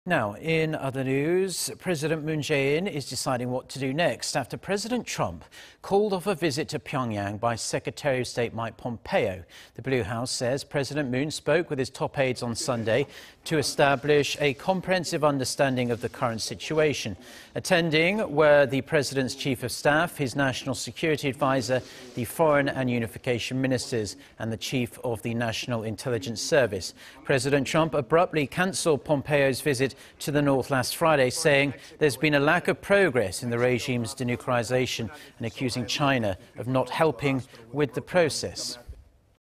President Moon deciding next moves after Trump canceled Pompeo's N. Korea visit
문대통령, 폼페이오 방북 무산 대책 논의
  President Moon Jae-in is deciding what to do next... after President Trump called off a visit to Pyongyang by Secretary of State Mike Pompeo.
  The Blue House says President Moon spoke with his top aides on Sunday to establish a comprehensive understanding of the current situation.
Attending were the President Moon's chief of staff, his national security adviser, the foreign and unification ministers and the chief of the National Intelligence Service.
 President Trump abruptly canceled Pompeo's visit to the North last Friday, saying there's been a lack of progress in the regime's denuclearization... and accusing China of not helping with the process.